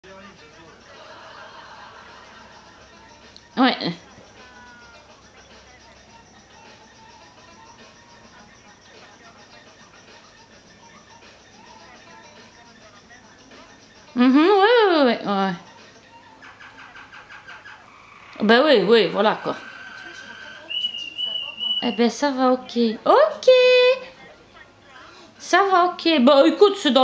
Moment calin entre Kenzo et Max
J'adore voir mes gros chiens , dans leurs moments tendresse